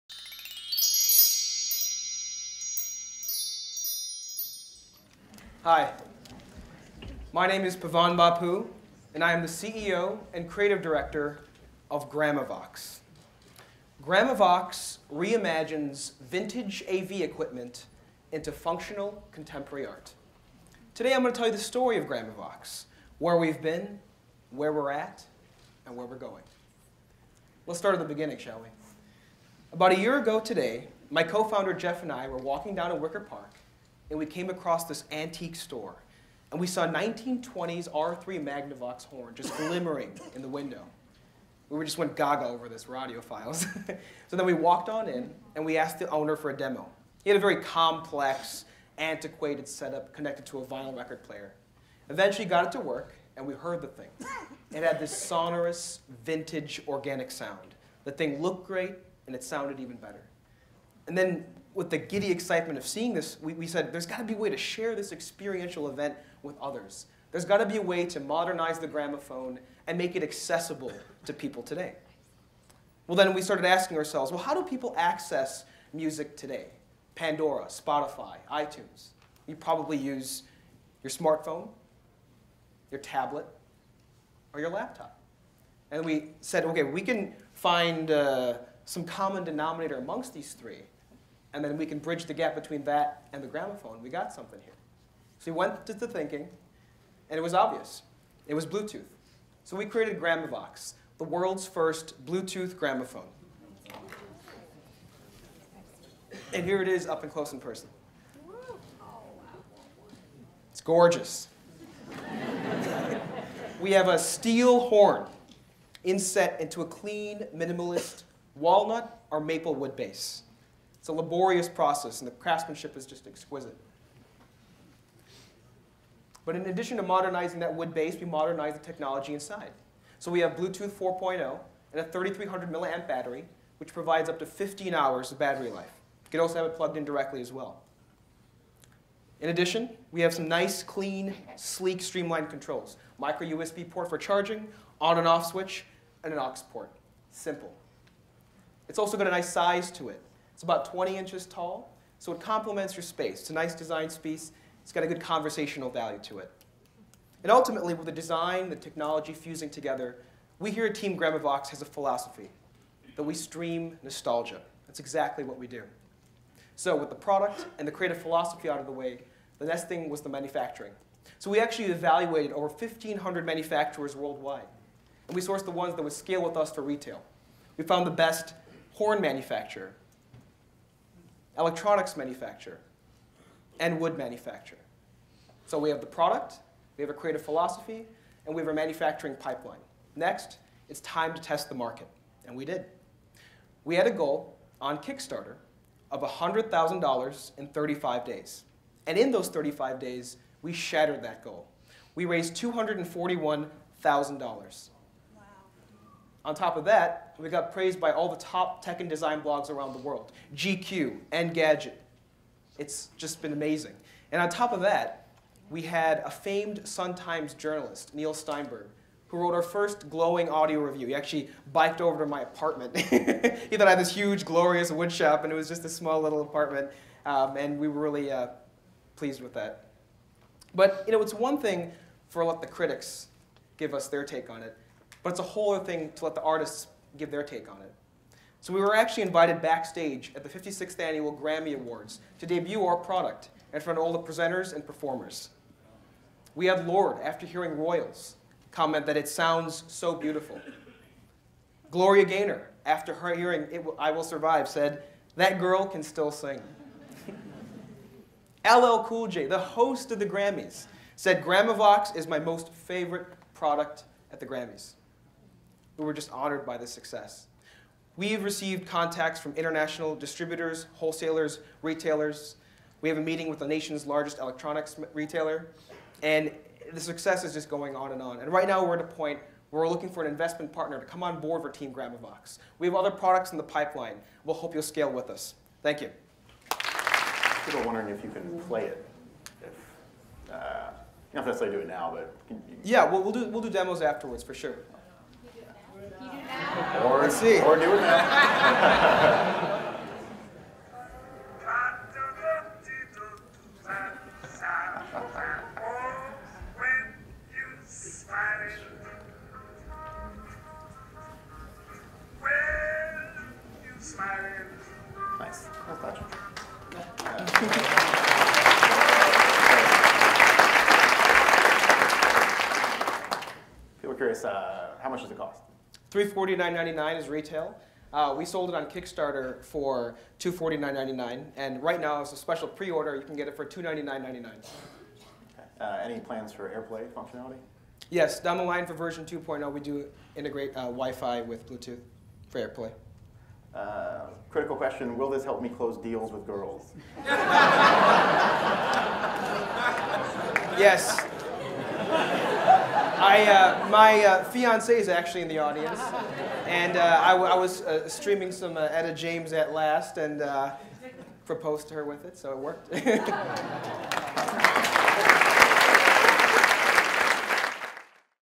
Gramovox Technori Pitch
CEO/CCO Pavan Bapu debuts Gramovox at the Technori Pitch on 02/25/14.